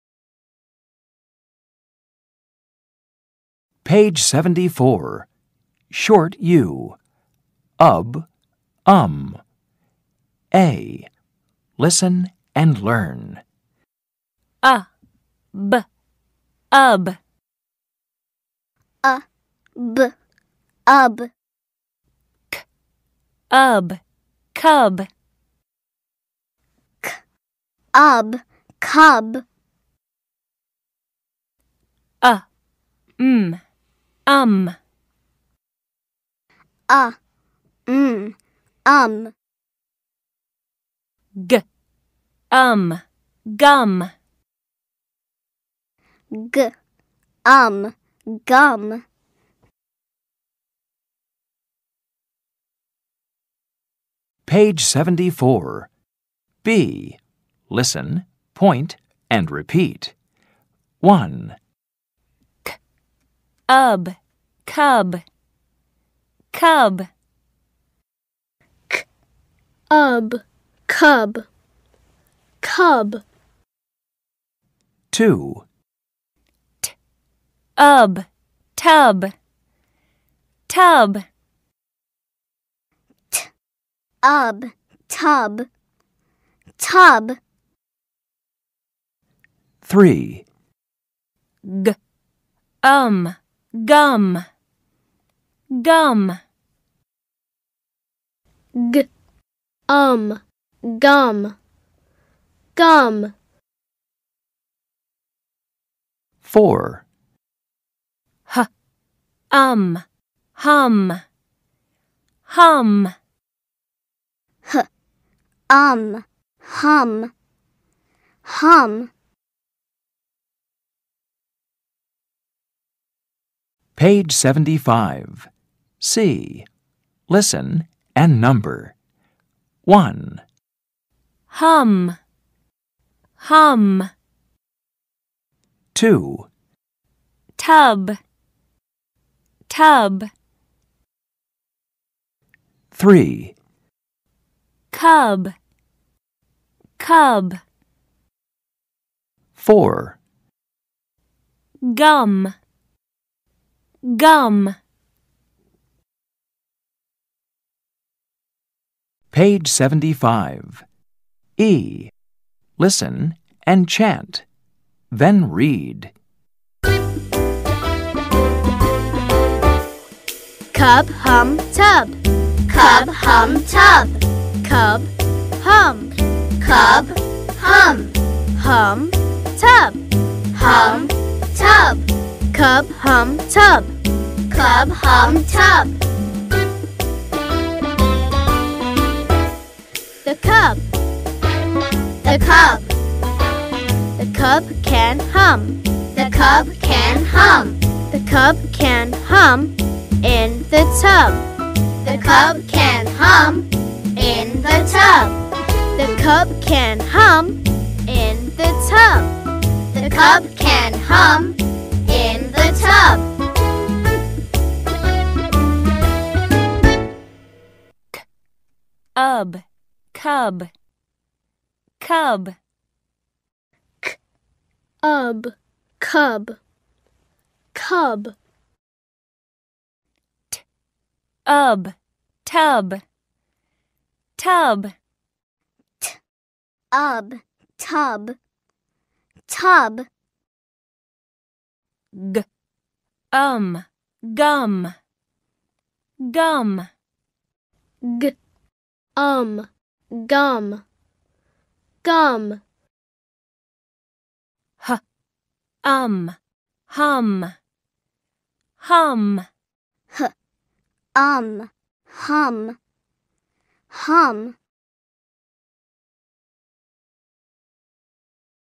Oxford Phonics World student book level 2 - short vowels - disc 2 - unit 8 - short u - ub um - cub
Oxford Phonics World student book level 2 - short vowels - disc 2 - unit 8 - short u - ub um - cub tub gum hum

OPW SB2 Disc2 Track53
OPW SB2 Disc2 Track54
OPW SB2 Disc2 Track55
OPW SB2 Disc2 Track56